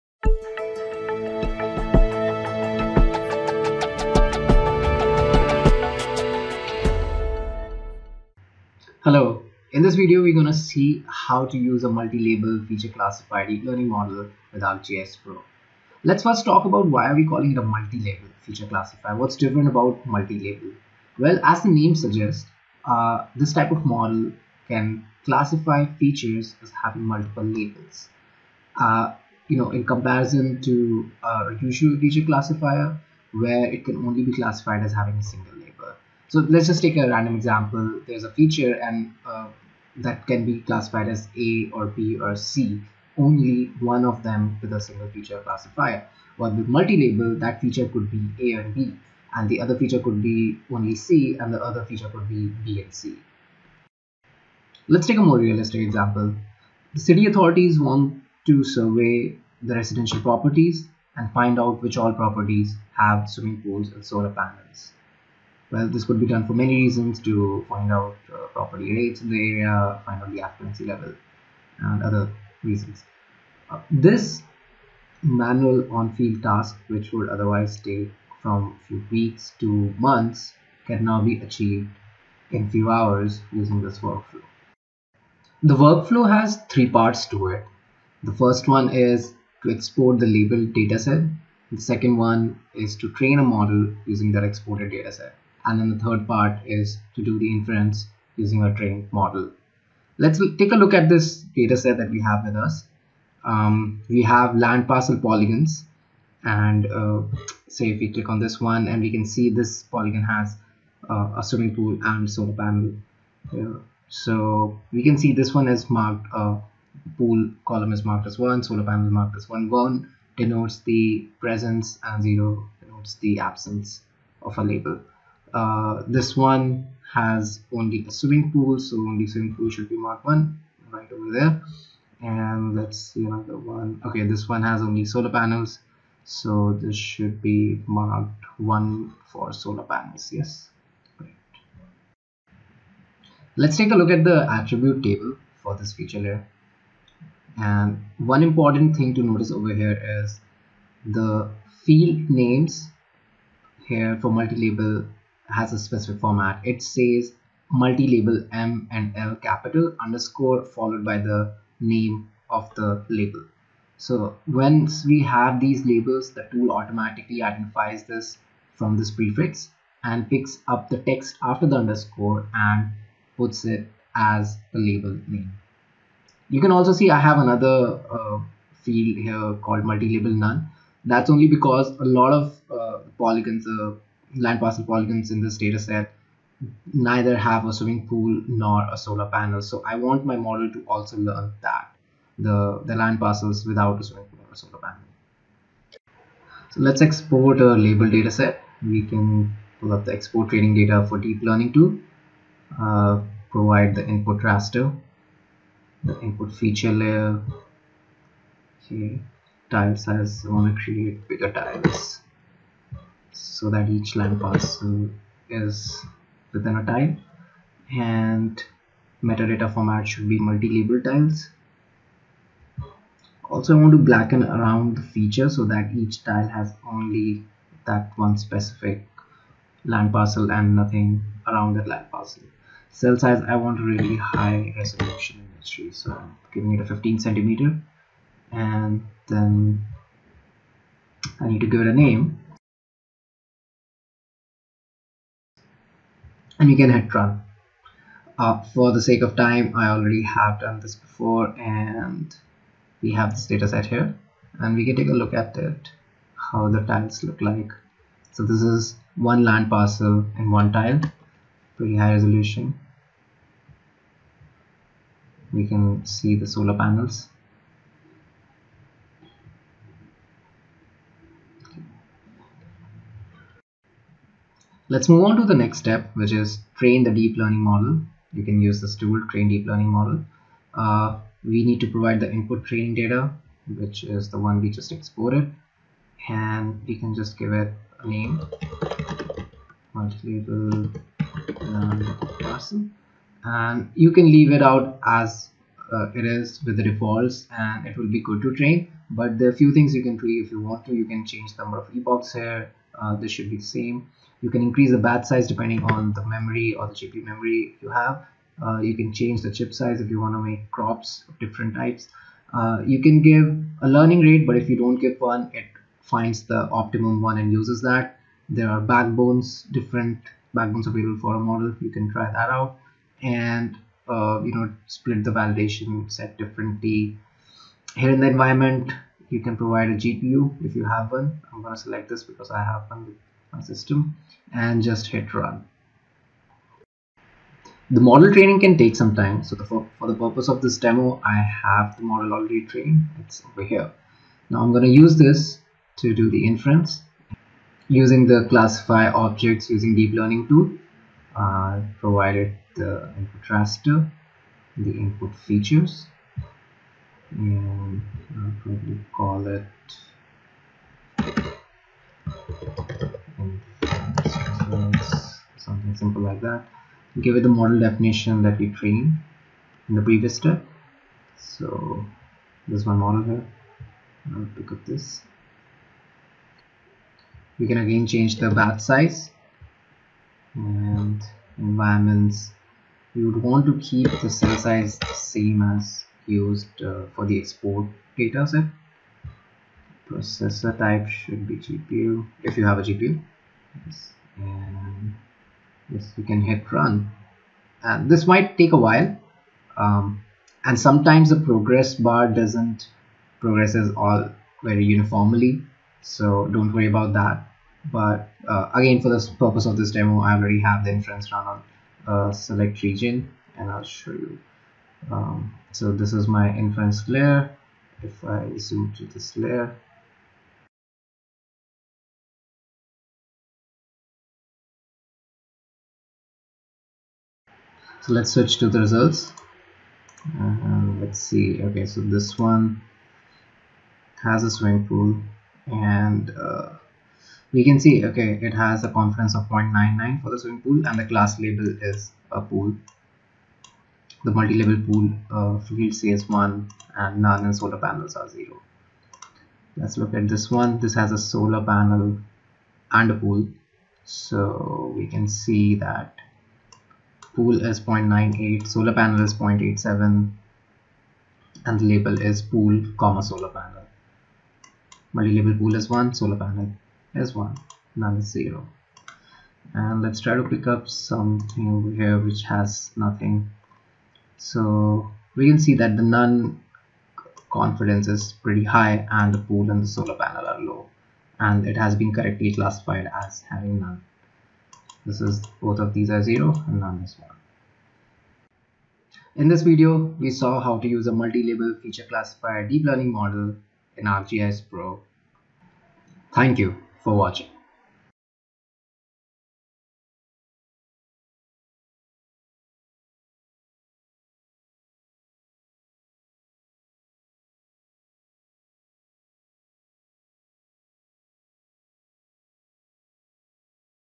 How To: Classify a Feature with Multiple Labels Using ArcGIS Pro
Learn how you can quickly classify a parcel that contains a pool, solar panels and more. ArcGIS Pro, and the ArcGIS Image Analyst extension, is used for all steps of the workflow.